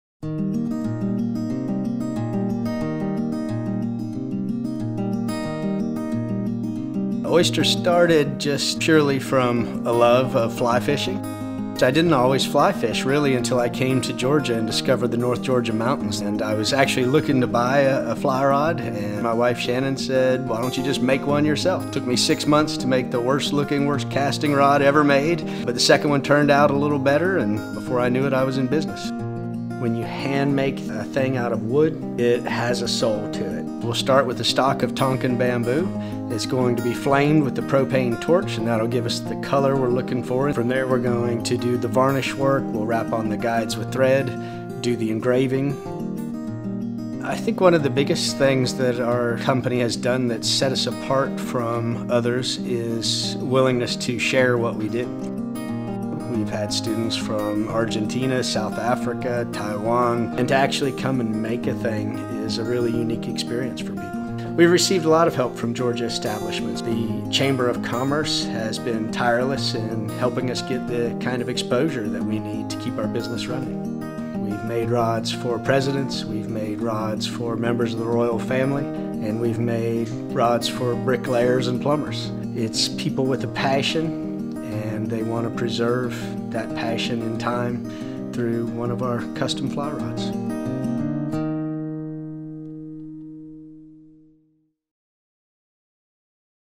Oyster Bamboo Fly Rods Georgia Small Business Rock Star of the Year
Oyster Bamboo Fly Rods Georgia Small Business Rock Star of the Year video that was played during the Georgia Small Business Rock Star of the Year ceremony of 2019.

Handcrafted for your lifetime…an heirloom for future generations.